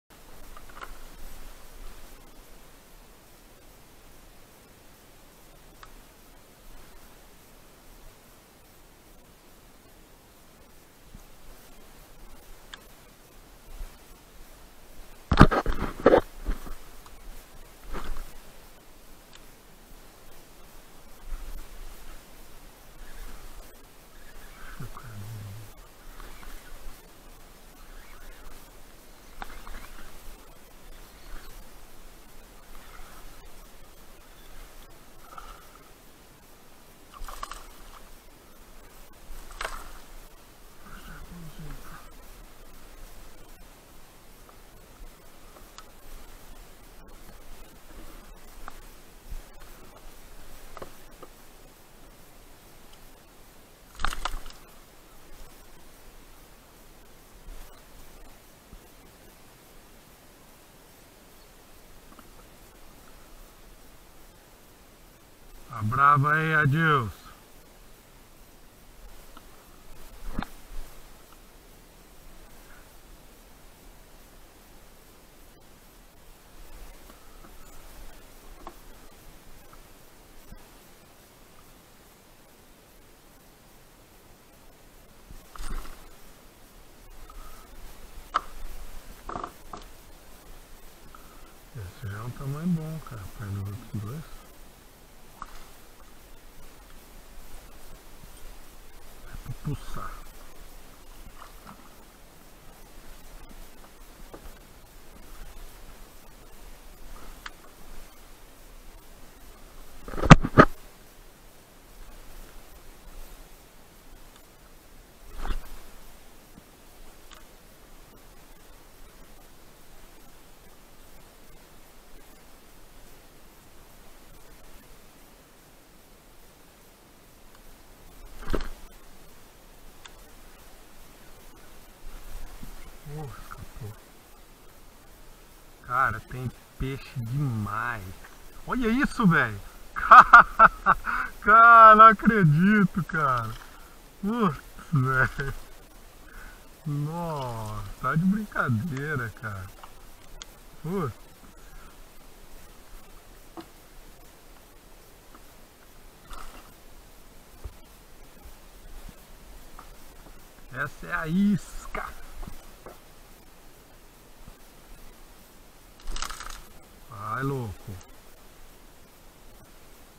Pescaria de Tucunaré em Piraju-SP
Pescaria de caiaque.
Materiais Utilizados:
Vara Daiwa Aird X 8-17 lbs
Carretilha Daiwa Fuego 100 xsl
Linha Multifilamento Maruri P.E. 0,20mm
Lider Fluocarbon Shock Leader Maruri 0,33mm
Vara Evolution Marine Sposts 8-15 lbs
Carretilha Venator S
Isca Brava 77 Marine Sports